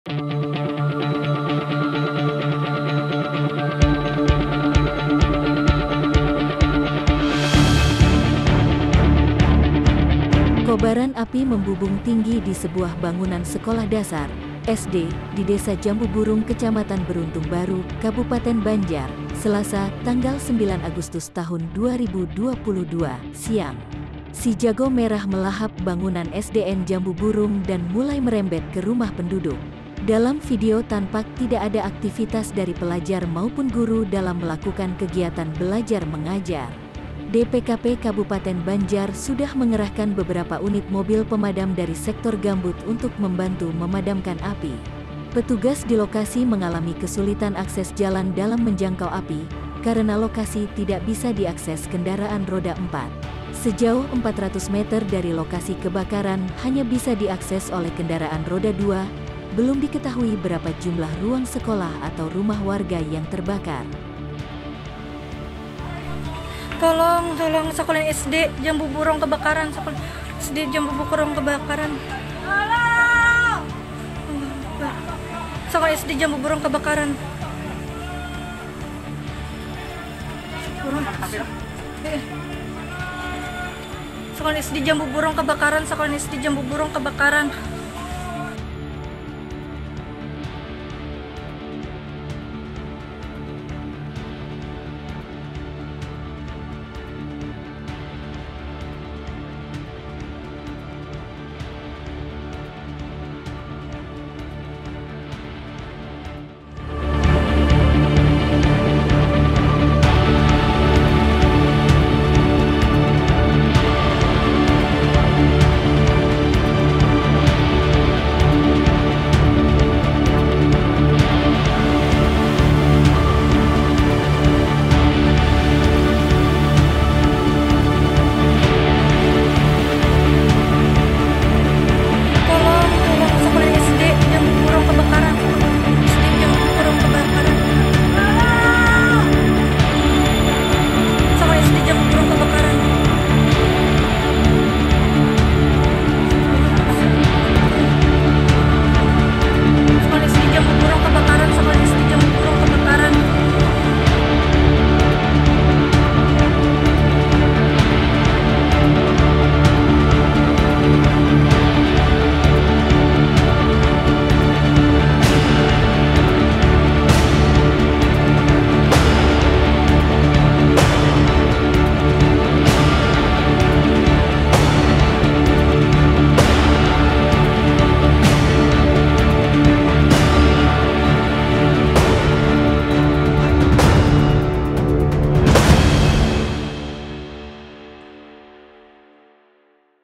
Gedung SDN Terbakar, Petugas Kesulitan Akses ke Lokasi
POS-KUPANG.COM - Si Jago Merah melahap bangunan SDN Jambu Burung dan mulai merembet ke rumah penduduk, Selasa 9 Agustus 2022 siang.

Kobaran api membubung tinggi di sebuah bangunan Sekolah Dasar (SD) di Desa Jambu Burung Kecamatan Beruntung Baru, Kabupaten Banjar. Selasa 9 Agustus 2021(POS-KUPANG.COM,Jocc)

VPO         : Jocc
Video  : Ig@terang_media